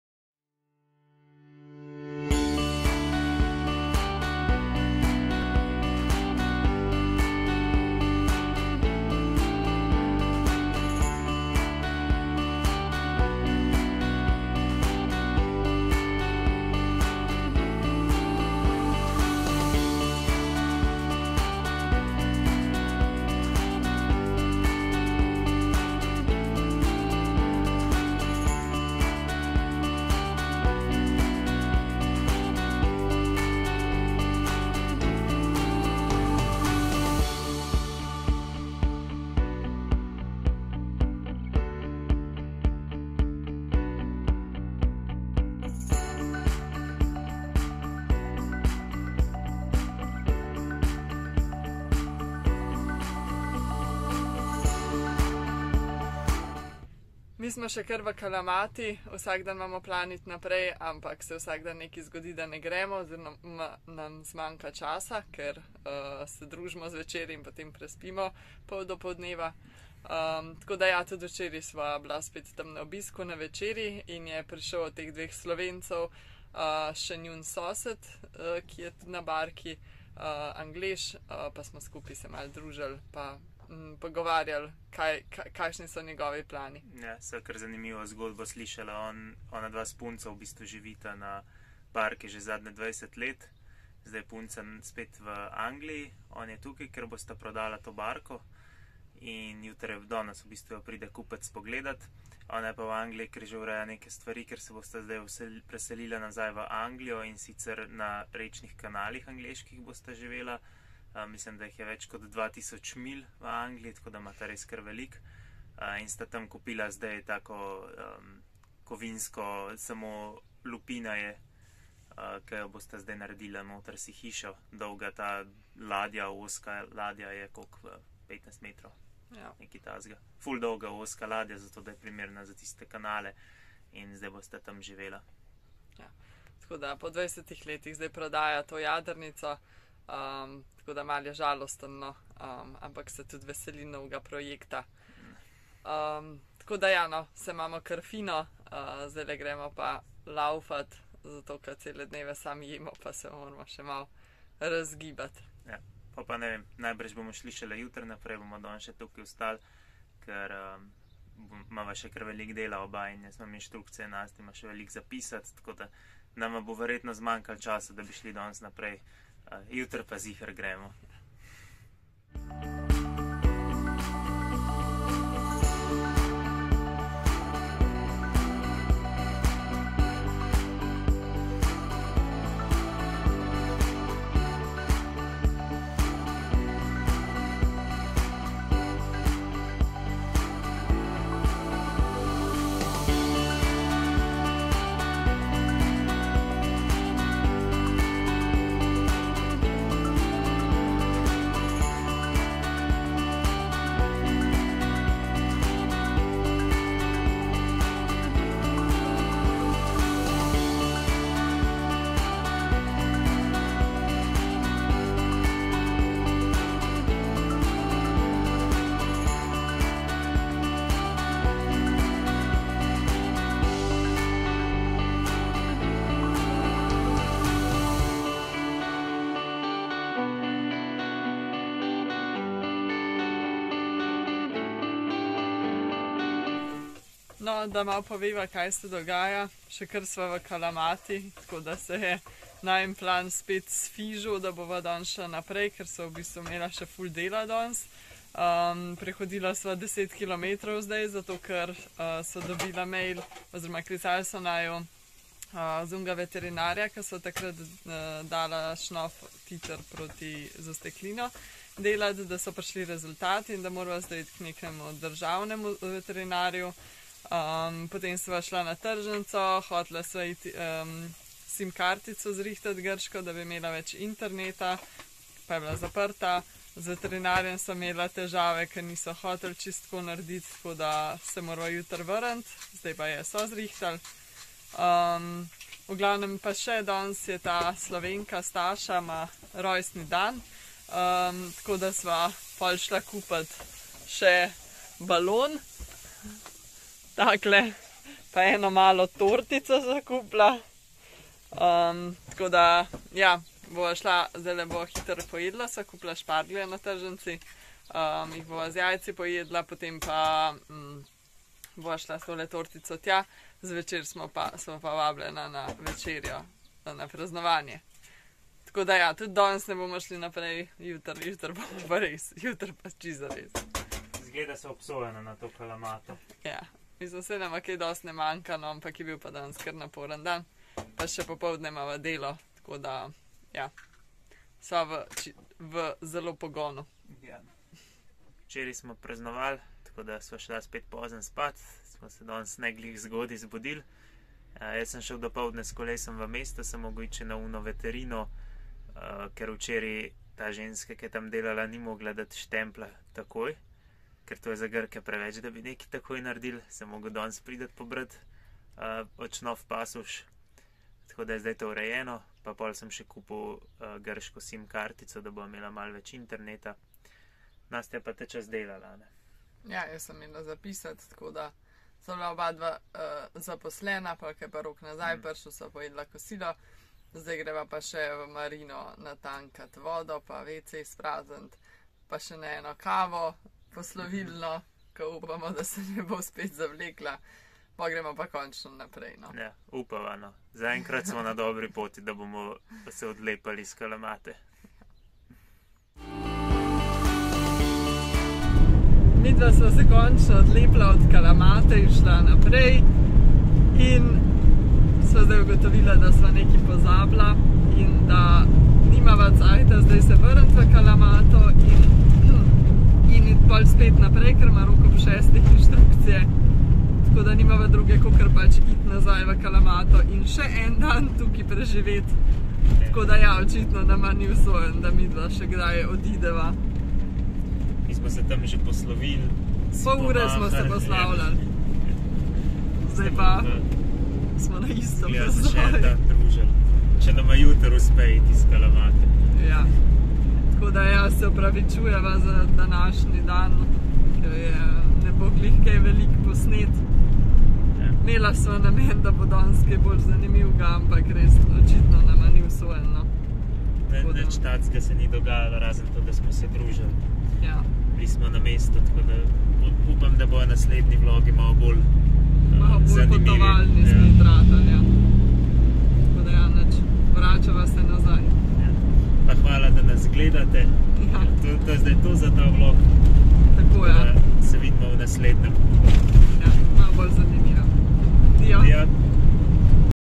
Zgodbe popotnikov | Potujoči vlog #65
➥ tvoje podpore bova zelo vesela, saj bomo lahko potovali še dlje!

Koristne informacije, ki ti znajo pomagati pri načrtovanju potovanja, najdeš na

Del potovanja je tudi spoznavanje podobno mislečih popotnikov in njihovih zgodb. V Kalamati se sicer nisva le družila z jadralci, ampak sva si vzela tudi čas za urejanje SIM kartice, za dogovarjanje z veterinarjem itd.

Imaš še kakšno vprašanje? Napiši ga v komentar!